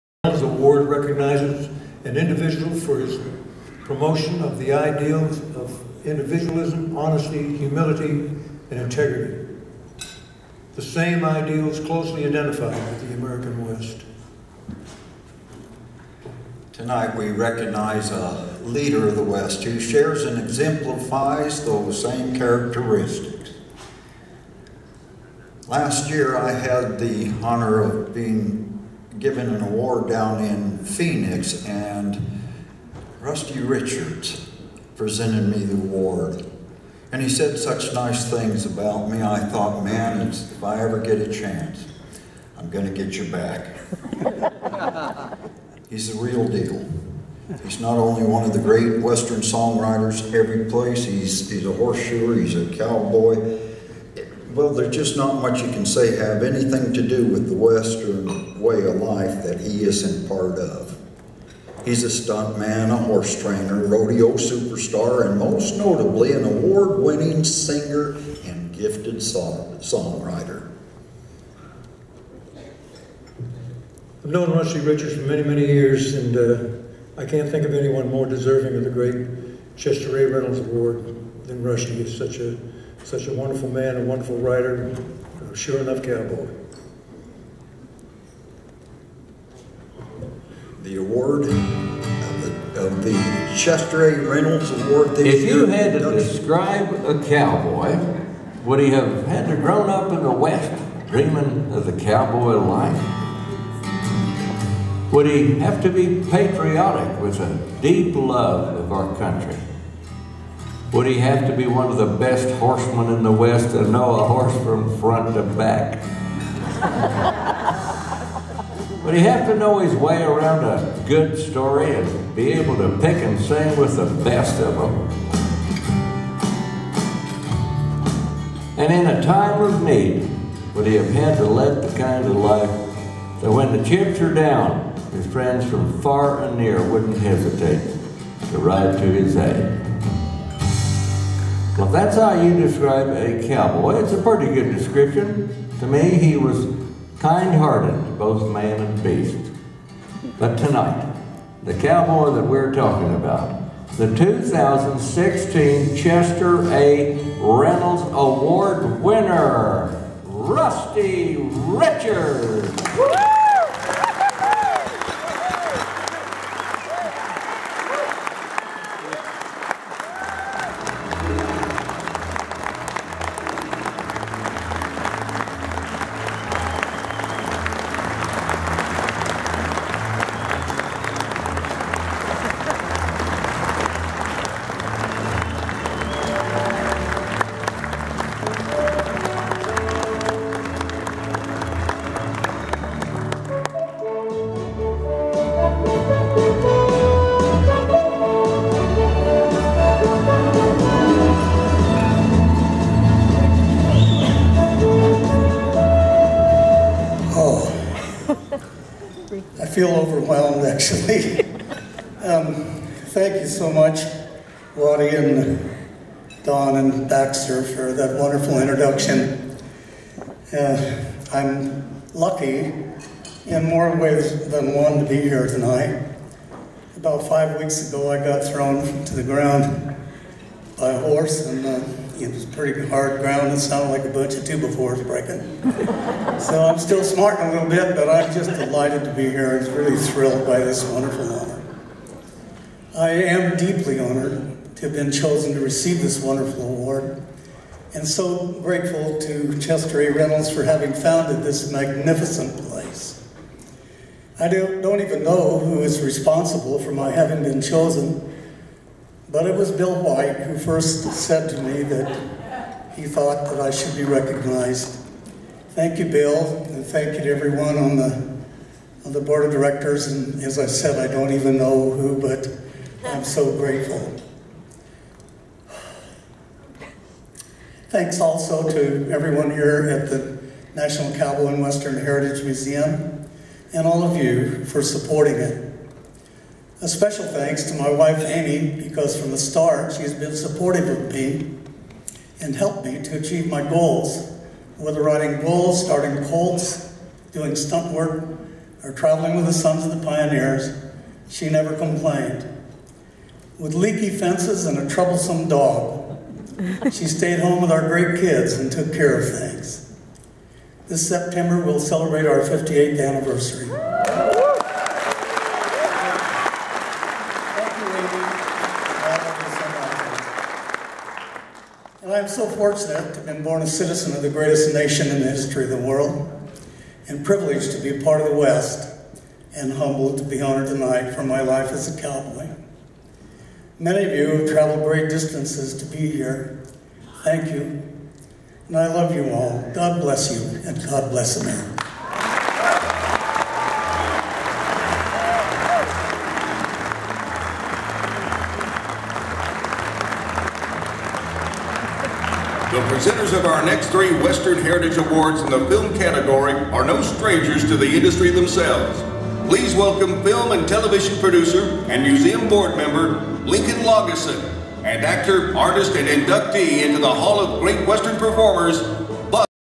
Rusty Richards at the National Cowboy and Western Heritage Museum
Rusty Richards receiving the Chester A. Reynolds Award at the National Cowboy and Western Heritage Museum in Oklahoma City.    April 16th 2016